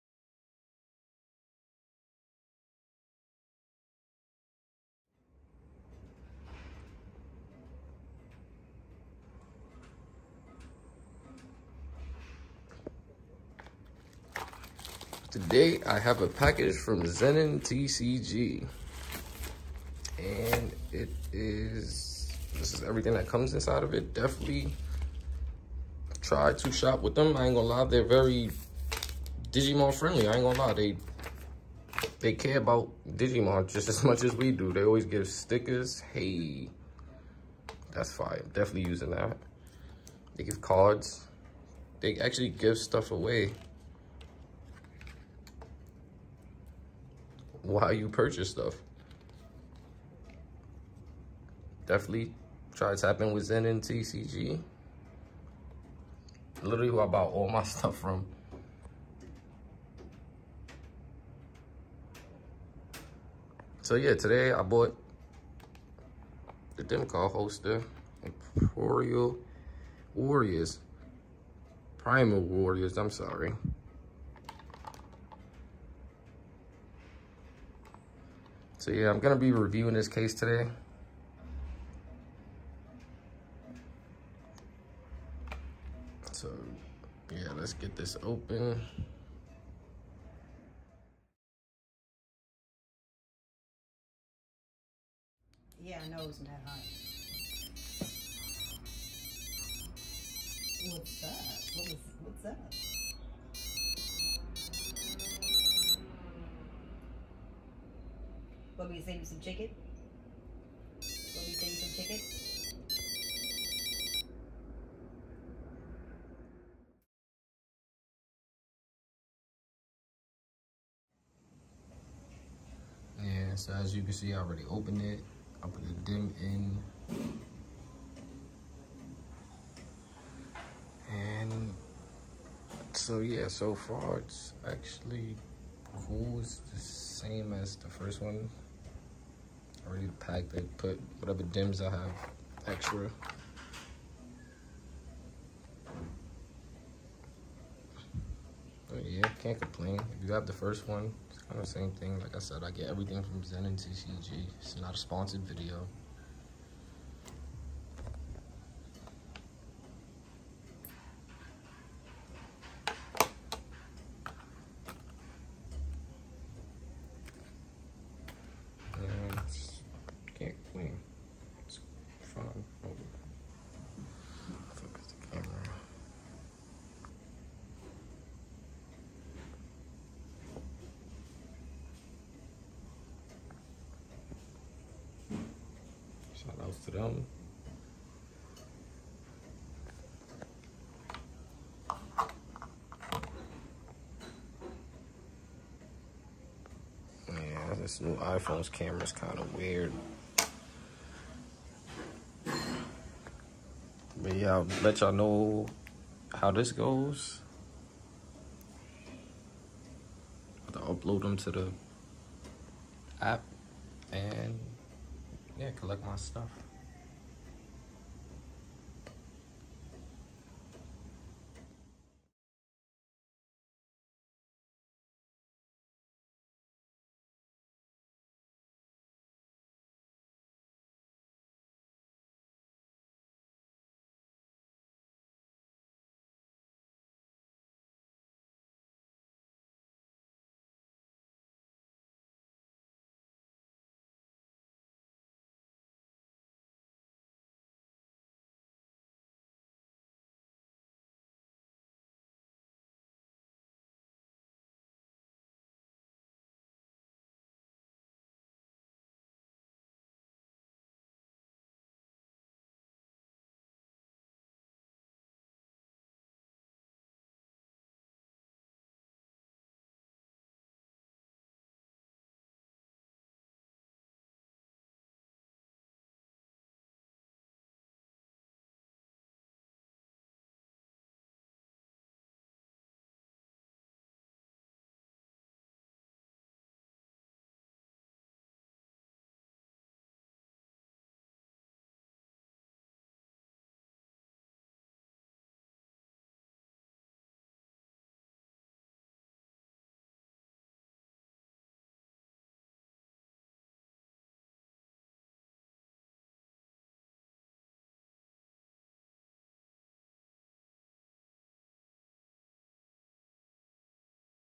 Digimon Vital Bracelet -Dim Card Holster Vol 2 - Primeval Warriors Dim Card Review
Opening and Reviewing a Digimon Vital Bracelet -Dim Card Holster Vol 2 - Primeval Warriors Dim Card